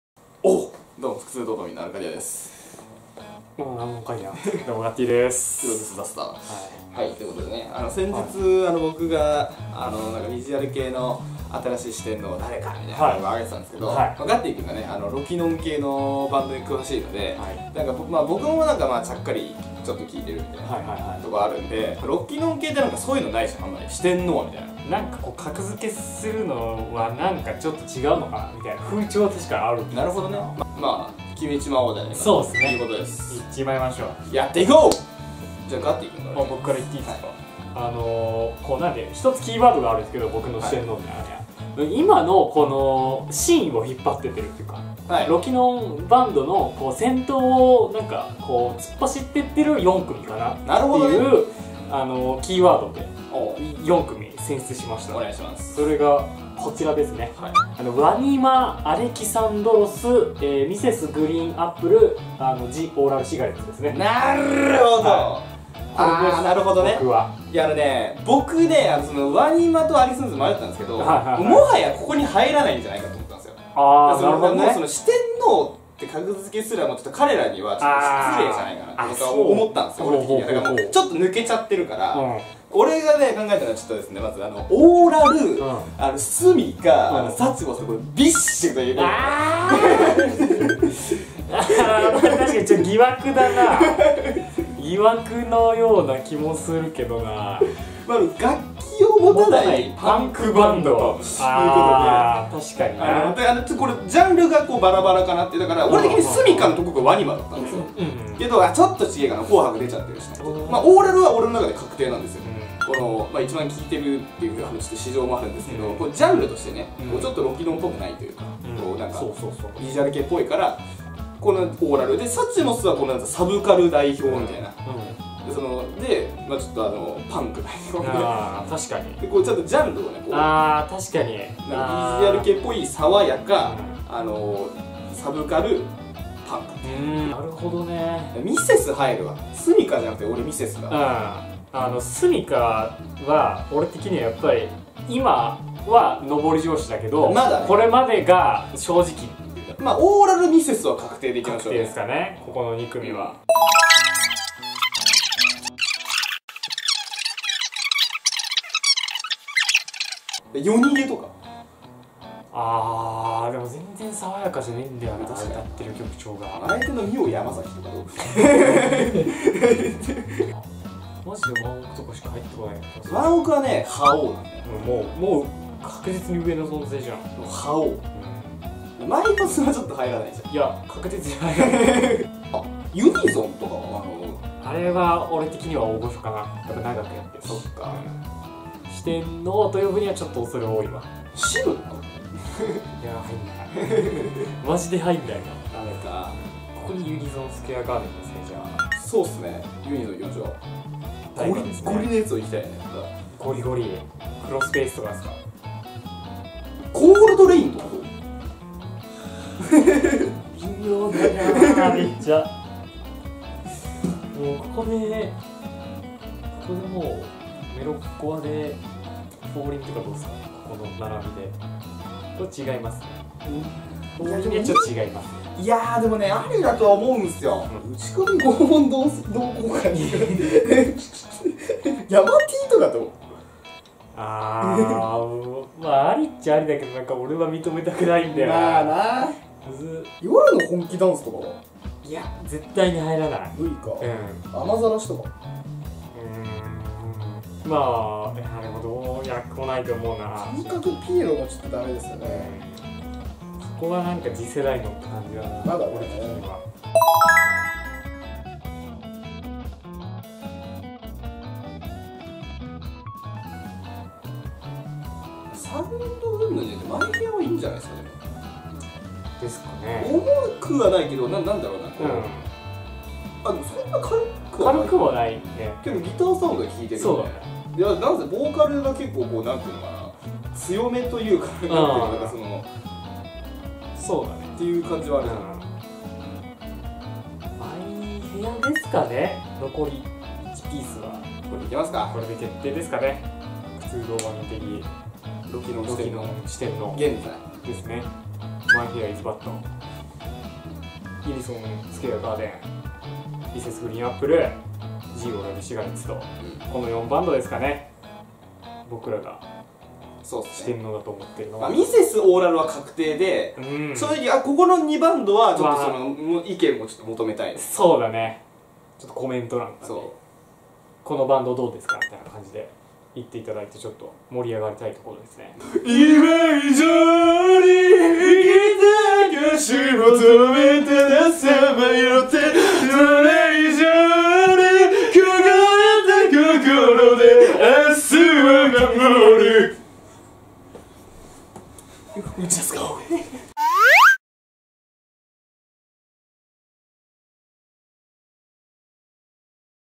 【雑談】ロキノンの四天王決めよう！
火曜、金曜、日曜週３更新！
他の曜日に短い動画、クソ茶番等も上げようと思ってます。

企画　アルカディア
編集　アルカディア

どうも腹痛ドーパミンです！
前回新世代V系四天王を予想したんですが、
今回はロキノン四天王！！
ロキノンはガッティー君がめっちゃ詳しいです。